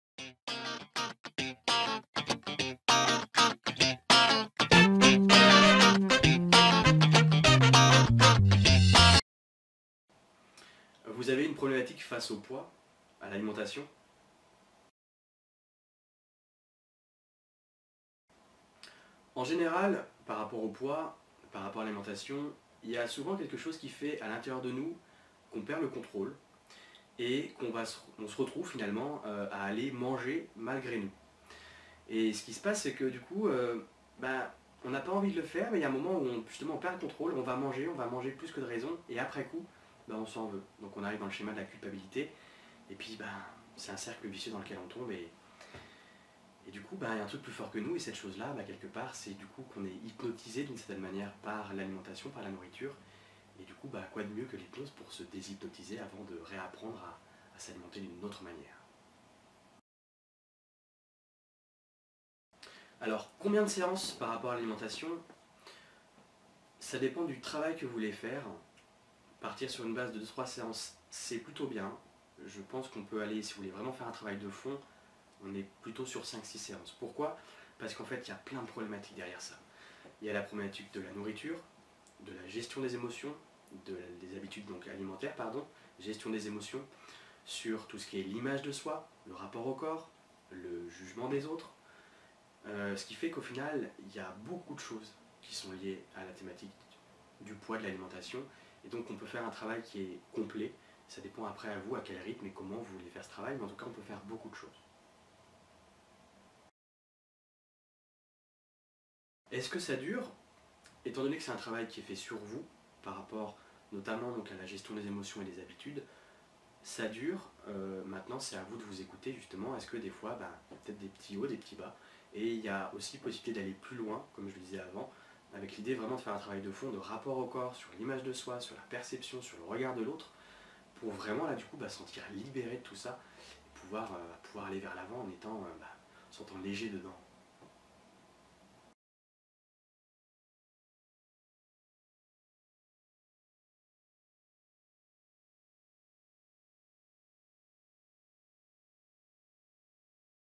Hypnose maigrir Paris - Hypnothérapeute Paris - Jérôme Saurin
Hypnose pour maigrir à Paris. Jérôme Saurin, hypnothérapeute à Paris évoque les principales questions liées à une séance d'hypnose pour perdre ou prendre du poids. Pourquoi l'hypnose, combien de séances ou encore la durée des effets dans le temps.